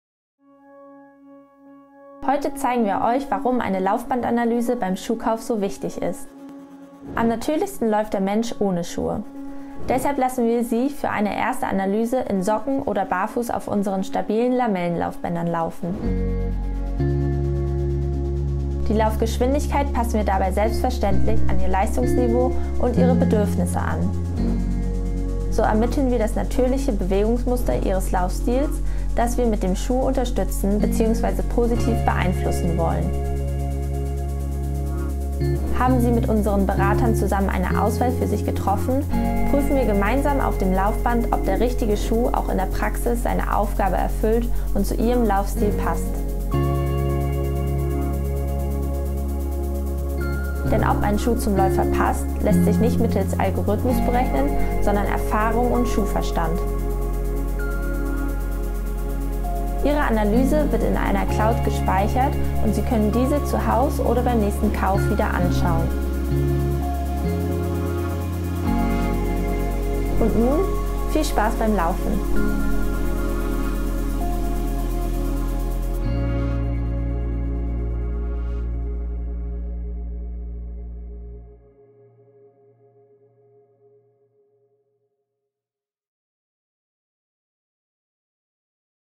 Lunge der Laufladen - Unsere Laufbandanalyse
Mit unserer Laufschuh-Beratung suchen wir für Dich den bestmöglichen Schuh. Die Laufbandanalyse ist dabei einer der wichtigsten Elemente. Sieh Dir unser neuestes Video an und komm gern selbst bald mal wieder zu uns in eine unserer Filialen.